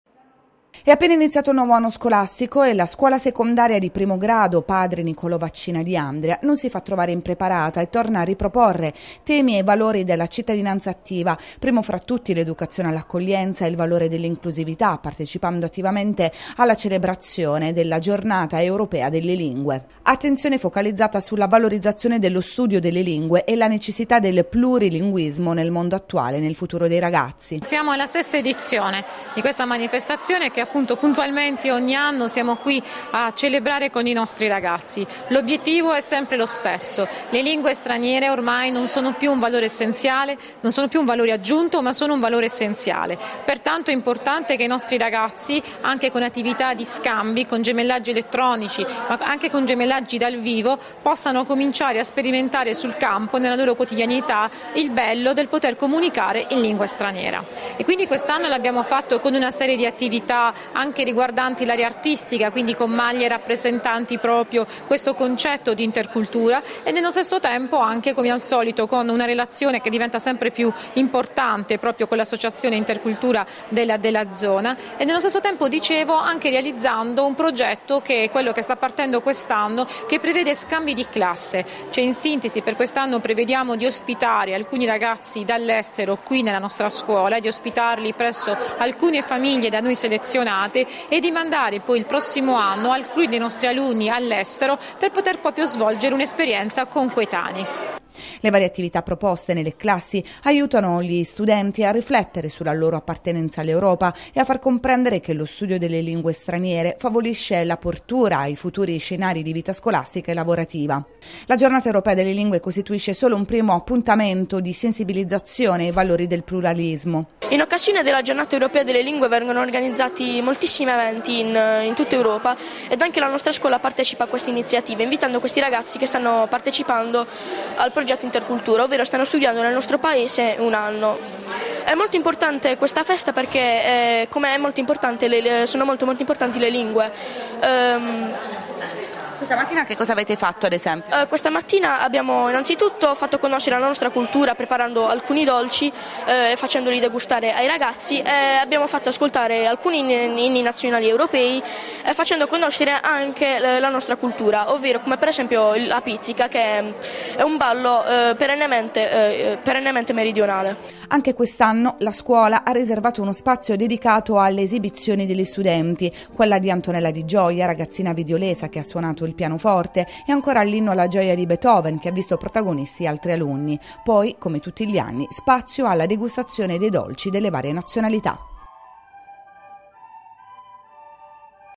Andria | La scuola “Vaccina” celebra la Giornata Europea delle Lingue
La scuola secondaria di primo grado “Vaccina” di Andria ha celebrato anche quest'anno la Giornata Europea delle Lingue.